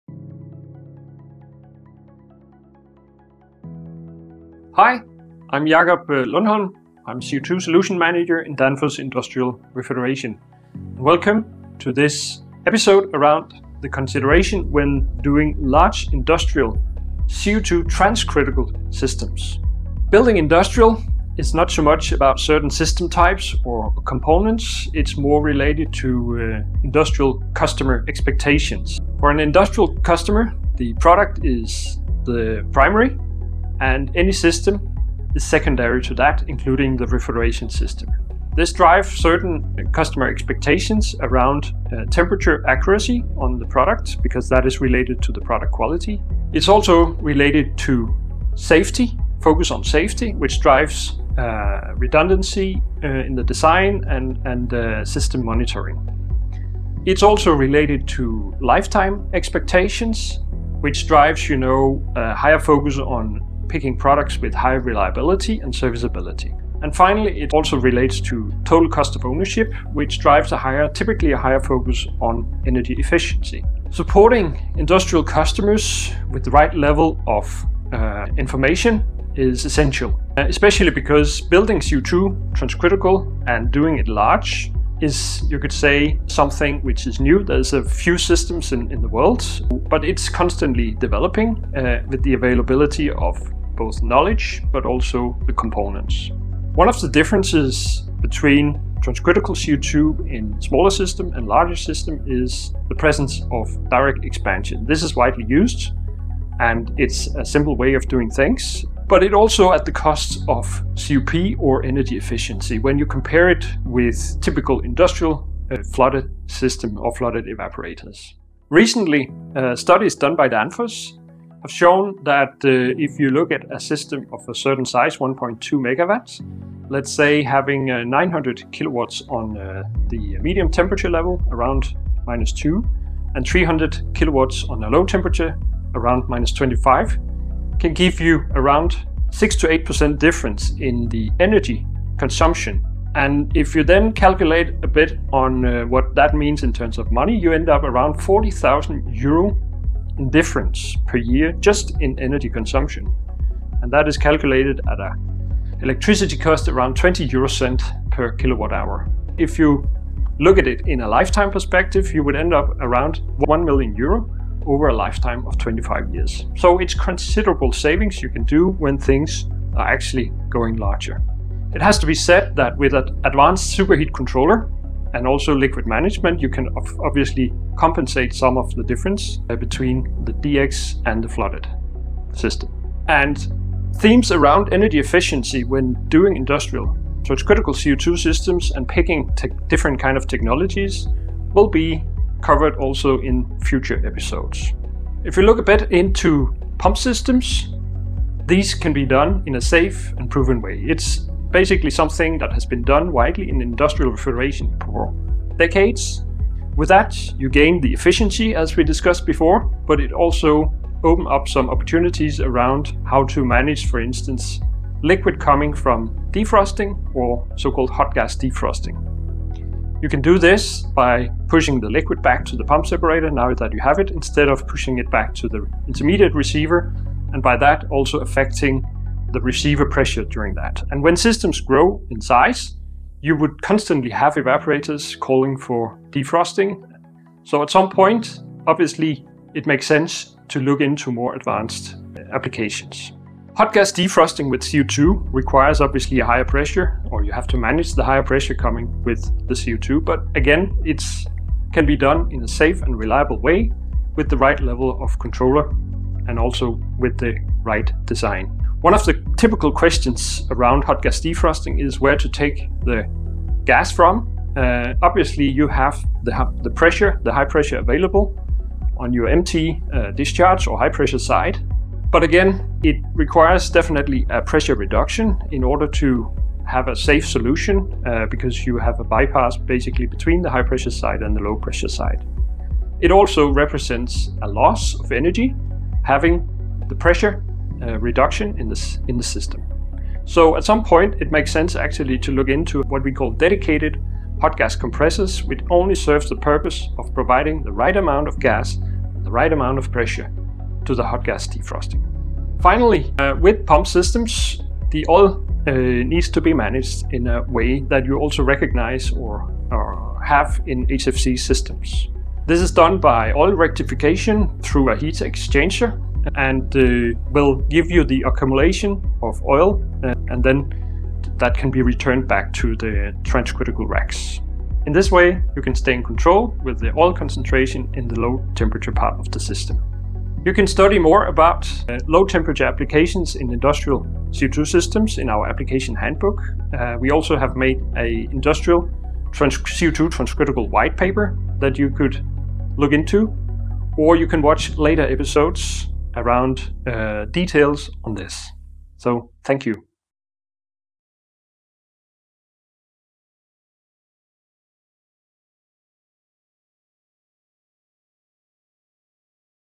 Building industrial CO₂ transcritical systems: key considerations
Join Jakob, our CO₂ Solutions Manager, as he unveils essential considerations for developing large-scale industrial CO₂ transcritical refrigeration systems.

Learn about energy savings when doing flooded systems and pump systems as a well proven solution and why beyond system types and components, understanding industrial customer expectations is crucial. Discover how advanced superheat controllers and liquid management techniques can mitigate the efficiency gap between DX and flooded systems, offering considerable savings and enhancing system performance in industrial settings.